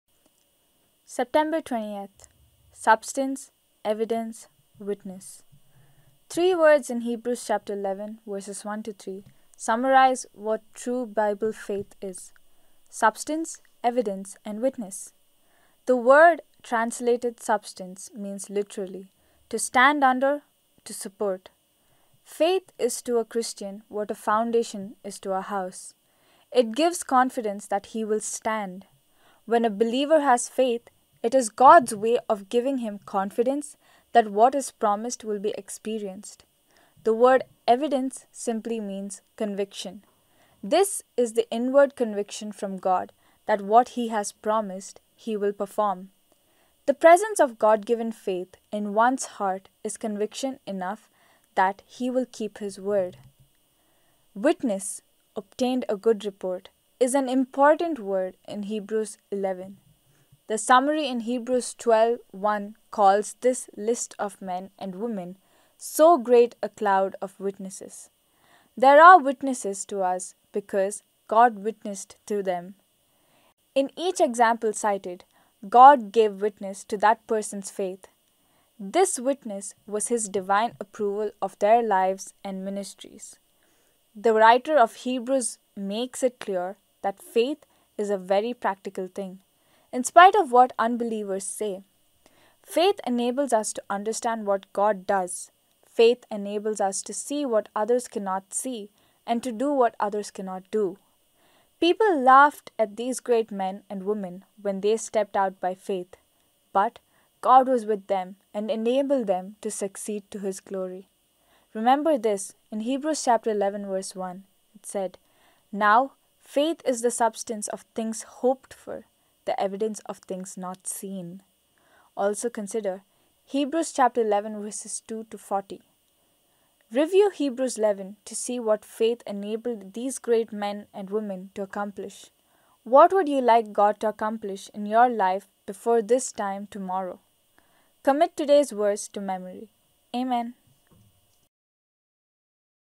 20 Sept English A Time To Be Renewed Warren Wiersbe
20 September

SUBSTANCE, EVIDENCE, WITNESS
Three words in Hebrews 11:1-3 summarize what true Bible faith is: substance, evidence, and witness. The word translated substance means literally “to stand under, to support.” Faith is to a Christian what a foundation is to a house: it gives confidence that he will stand. When a believer has faith, it is God’s way of giving him confidence that what is promised will be experienced.

The word evidence simply means “conviction.” This is the inward conviction from God that what He has promised, He will perform. The presence of God-given faith in one’s heart is conviction enough that He will keep His Word.

Witness (“obtained a good report”) is an important word in Hebrews 11. The summary in Hebrews 12:1 calls this list of men and women “so great a cloud of witnesses.” They are witnesses to us because God witnessed to them. In each example cited, God gave witness to that person’s faith. This witness was His divine approval of their lives and ministries.

The writer of Hebrews makes it clear that faith is a very practical thing, in spite of what unbelievers say. Faith enables us to understand what God does. Faith enables us to see what others cannot see and to do what others cannot do! People laughed at these great men and women when they stepped out by faith, but God was with them and enabled them to succeed to His glory.

Remember this: “Now faith is the substance of things hoped for, the evidence of things not seen” (Hebrews 11:1).
Also consider: Hebrews 11:240.
Action assignment: Review Hebrews 11 to see what faith enabled these great men and women to accomplish. What would you like God to accomplish in your life before this time tomorrow? Commit today’s verse to memory.